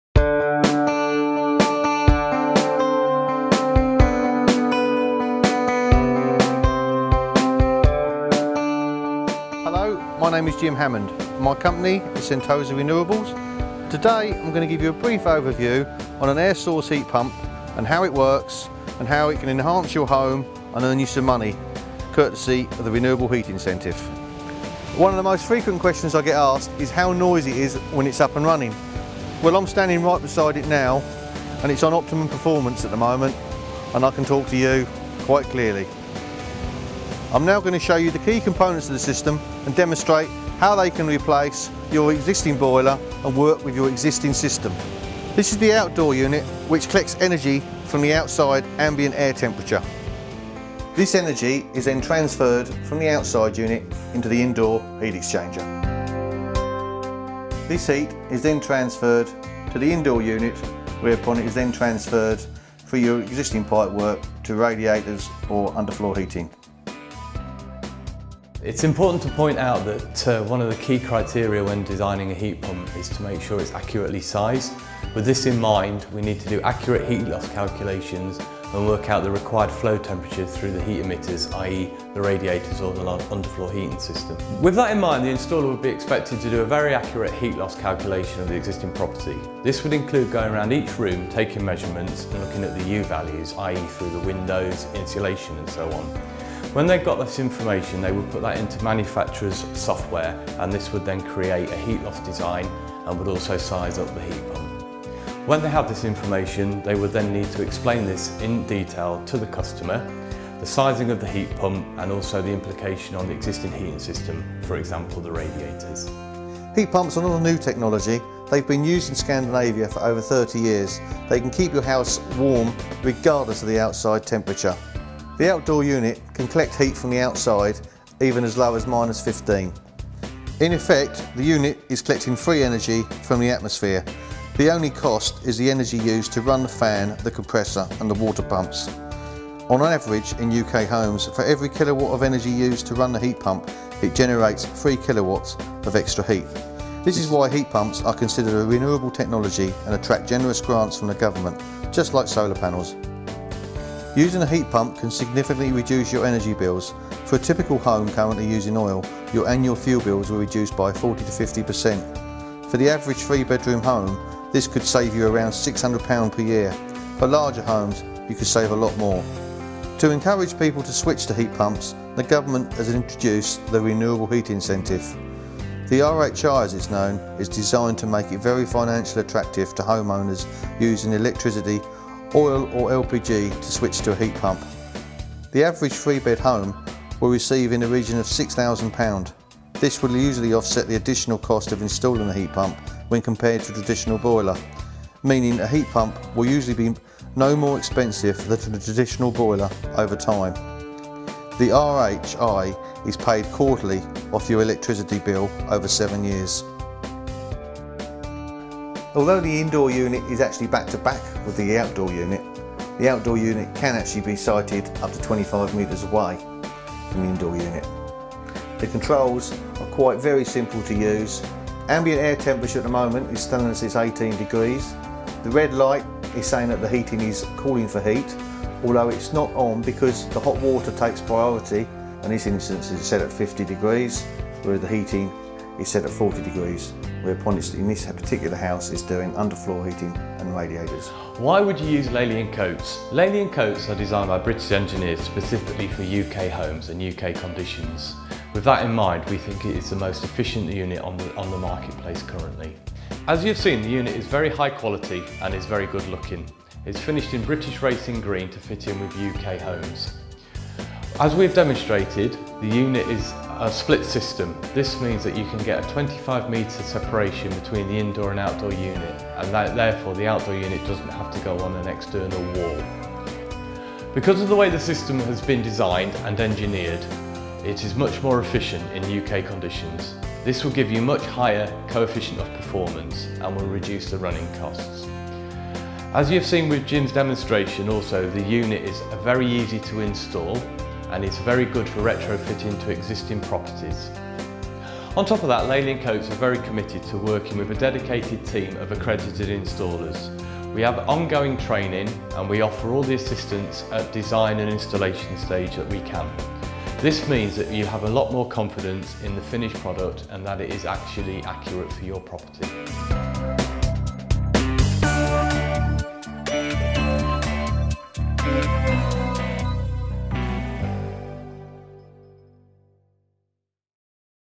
Sentosa Renewables Ltd
How you can significantly reduce your heating bills with a heat pump by Sentosa Renewables  based in  Essex . MCS accredited allowing customers to receive Renewable Heat Incentive from the government